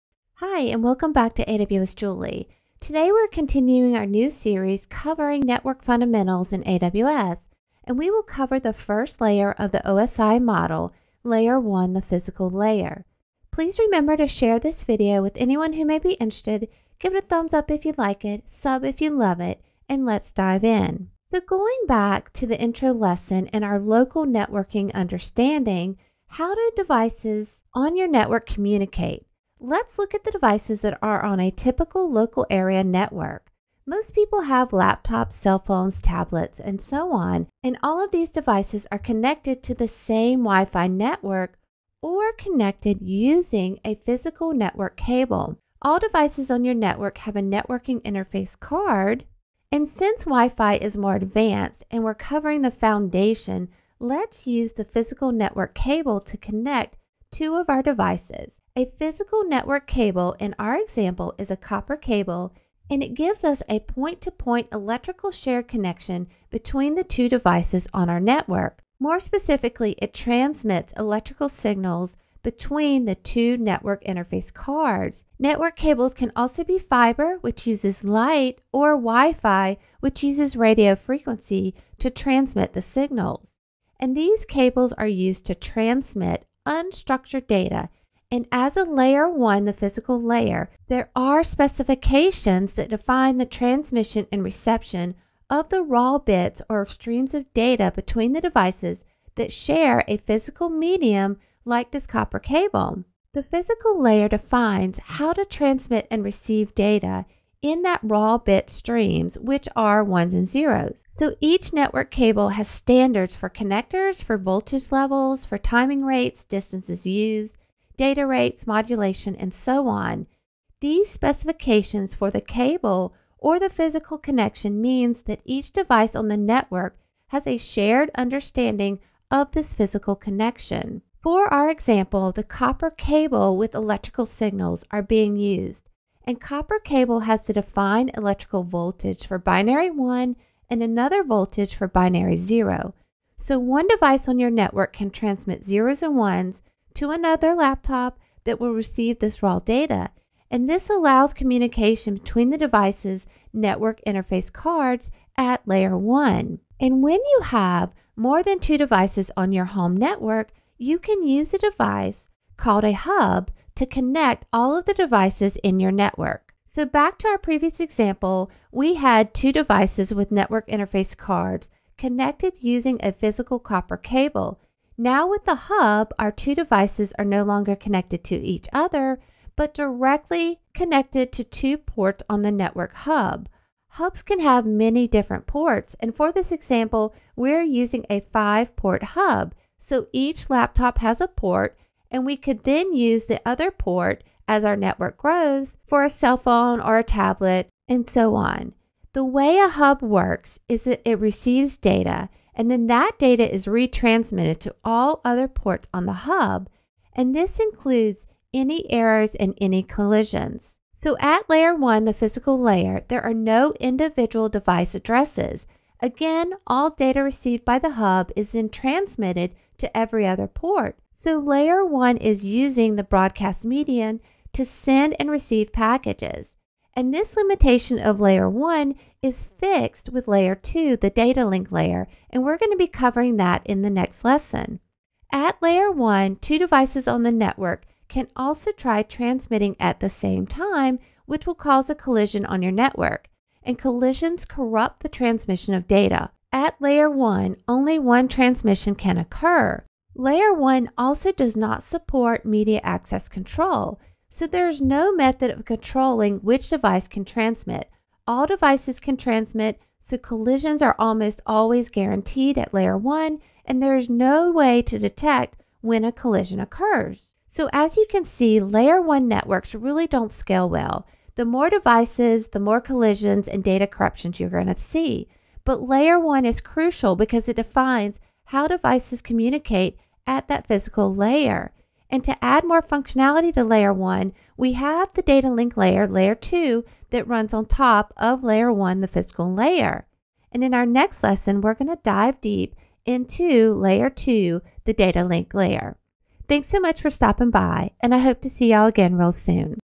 Networking Fundamental in AWS - Layer 1- physical layer of the OSI Model
Hi and welcome back to AWS Julie, today we are continuing our new series covering network fundamentals in AWS, and we will cover the first layer of the OSI model, layer 1 the physical layer.